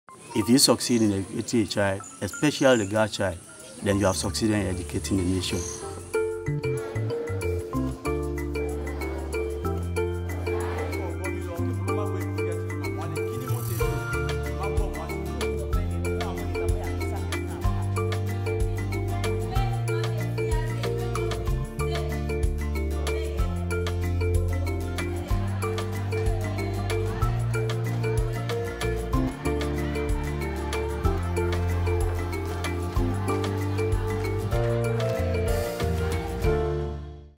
Changing attitudes with human rights education
Thanks to our supporters 25,000 women and girls have taken part in programmes to teach them about their rights and to take a stand against harmful practices like FGM and early forced marriage.